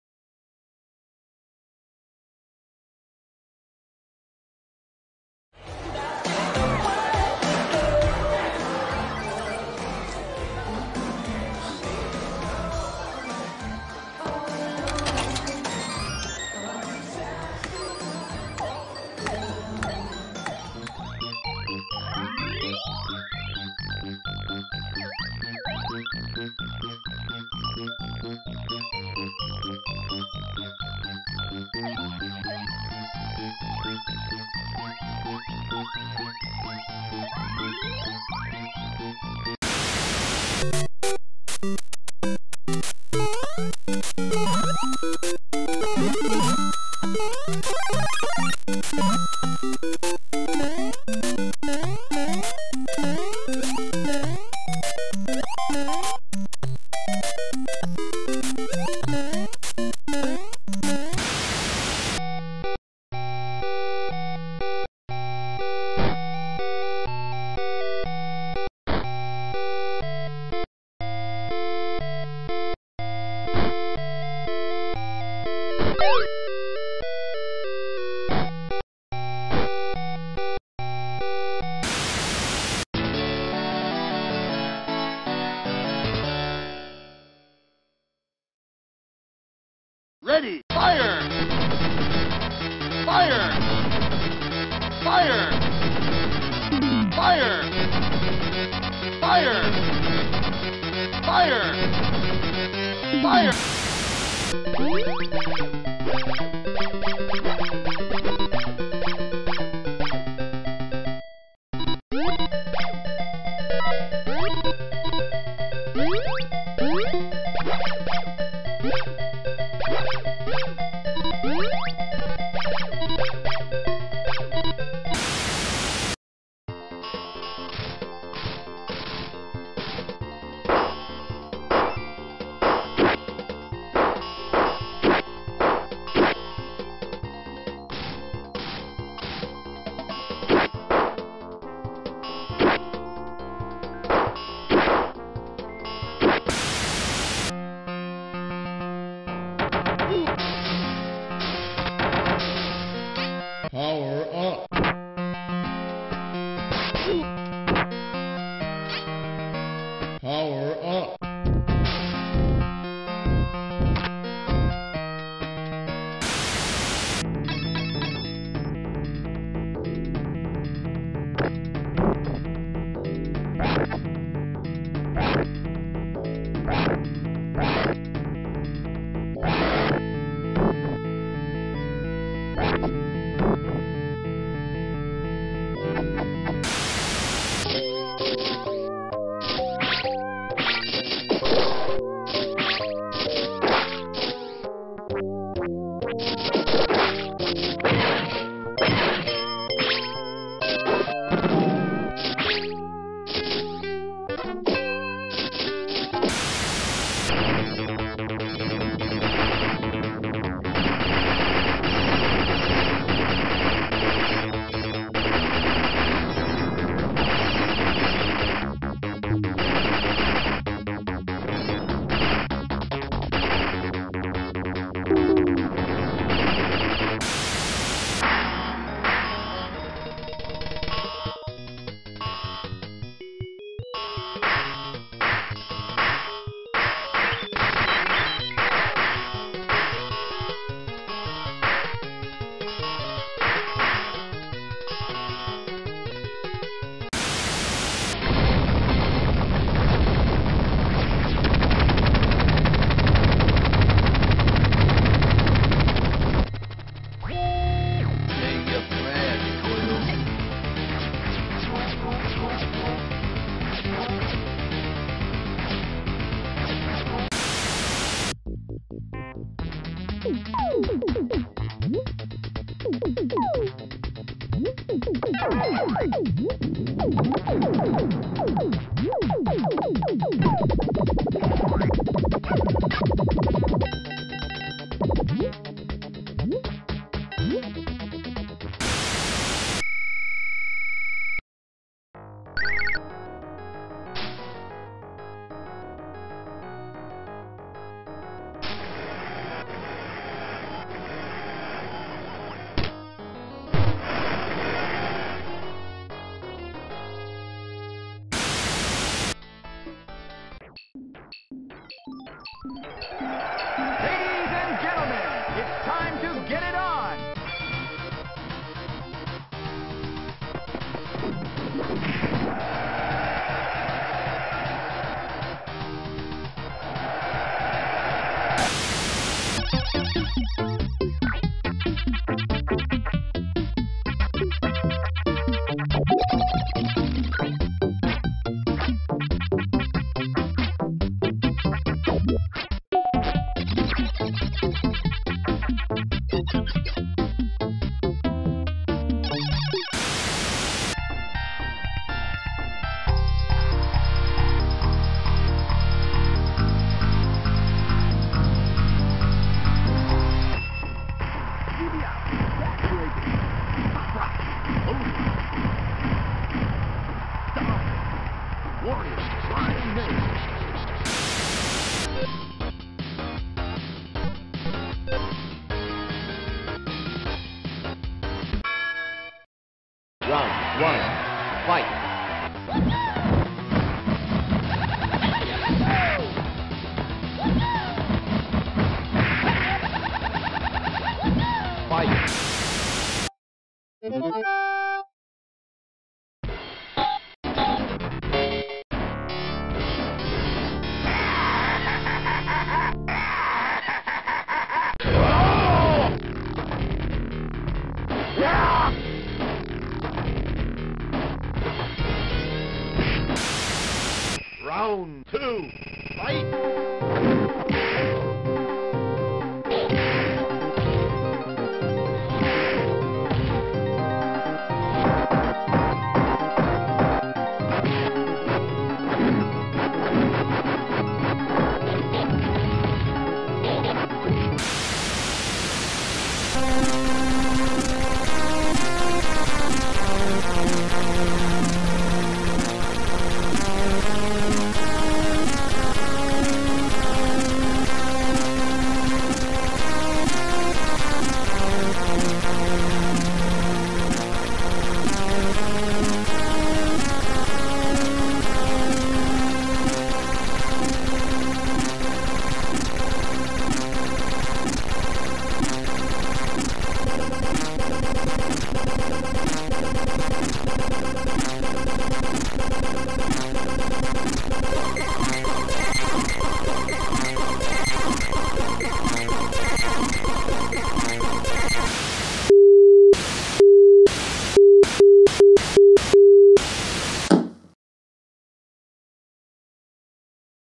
Arcade Games 80/90s Tribute (Personal selection 20 games)
Tribute to the arcade games of the 80/90s. Video includes 20 titles selected by me.